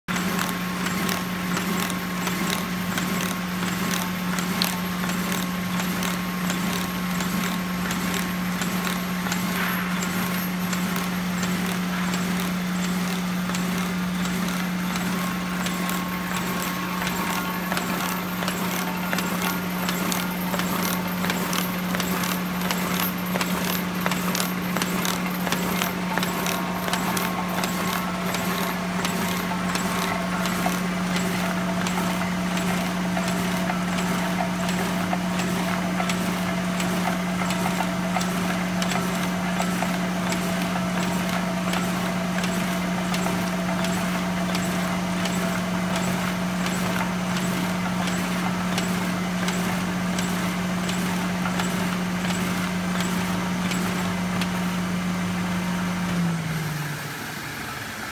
Horizon used jogger PJ-77 - LA-POSTPRESS - #22064
offer consists of:

Horizon PJ-77
jogger receiving tray

- sheet size: max. 470x318mm; min. 210x148mm
- pile height: max. 90mm
- sink: max. 30mm

- dimensions: 490x400x460mm
- weight: 40kg

We can also organize the following for you:

packaging, loading, transport ( by ship or plane) including customs